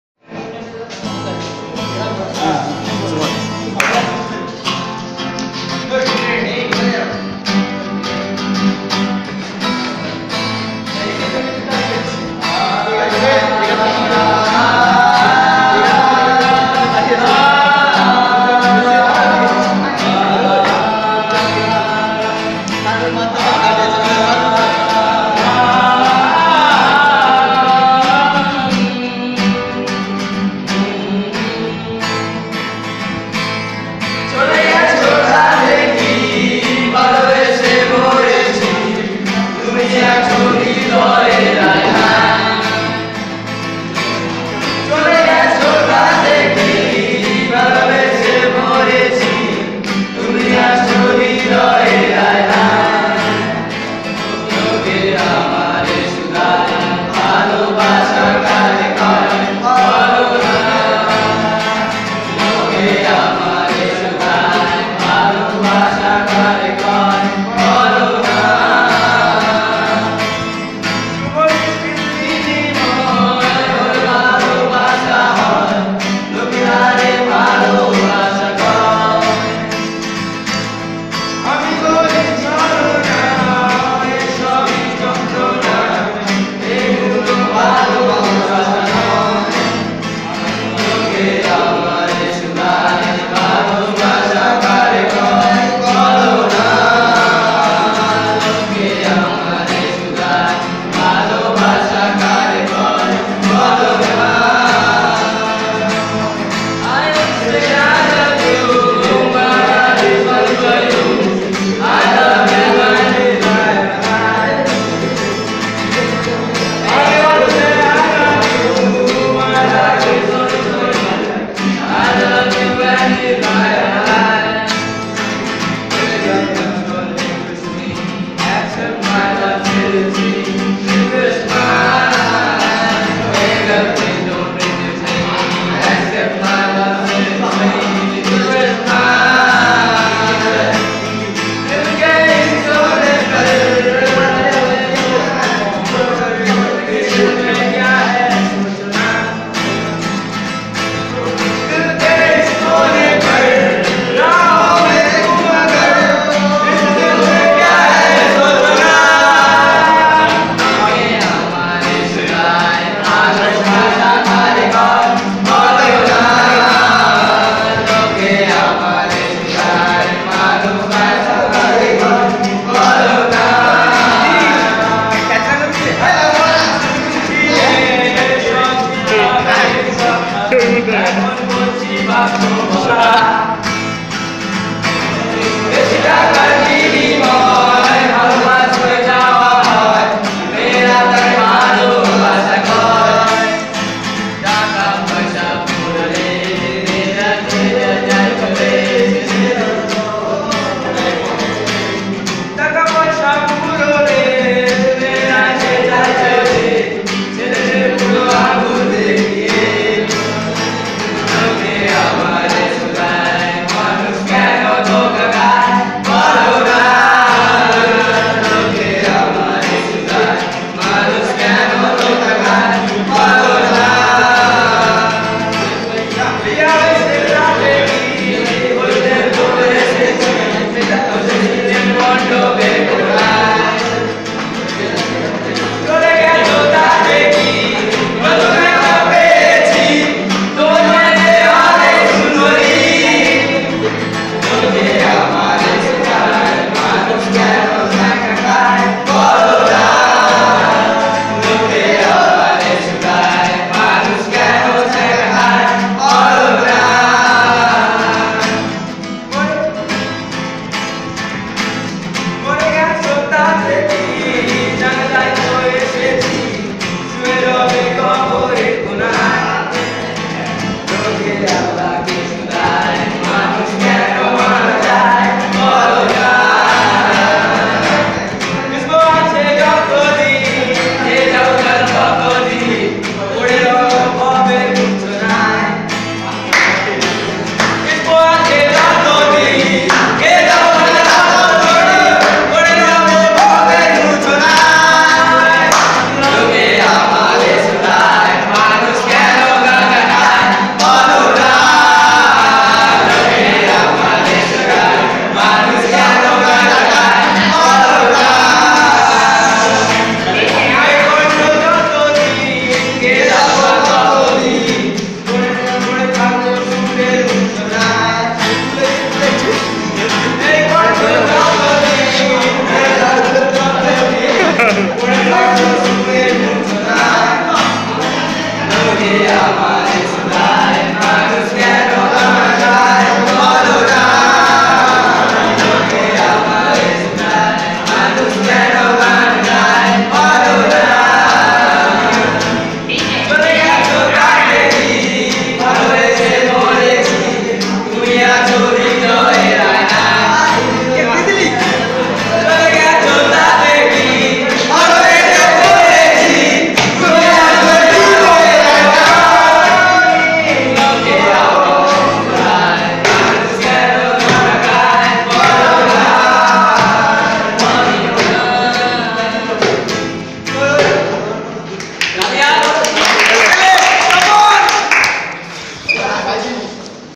H.G.S.V.V school rocker boys
H.G.S.V.V school rocker boys .with upcoming giterist- Soumyajit samanta